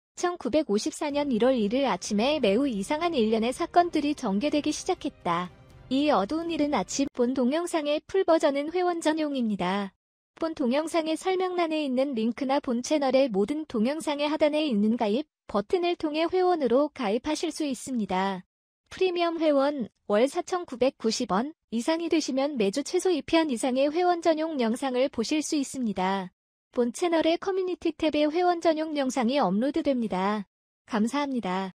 [회원용][충격] 톨화이트 노르딕 외계인과 우주에서 샤워와 식사를 한 남자의 외계인 납치 체험담!
본 영상은 회원전용으로, 총 길이 5분20초입니다.
외계인의 UFO에 납치되어 샤워와 식사를 한 사람의 구체적인 체험담을 담고 있습니다.
본 채널의 프리미엄 회원으로 가입하여 혜택을 누려보세요.